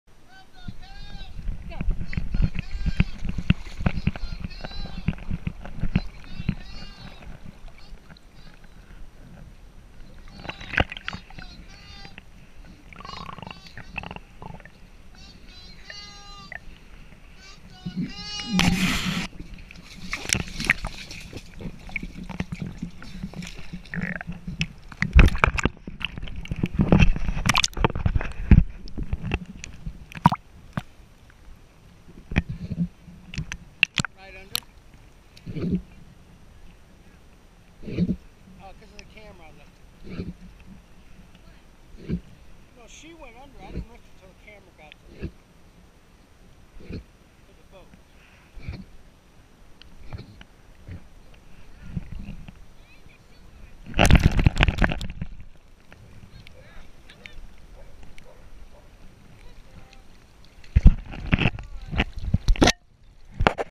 Pasha capsized boat
Newfoundland Dog named Pasha performing the WRDX, Water Rescue Dog Excellent, rescues from capsized boat exercise with Go Pro camera attached to her back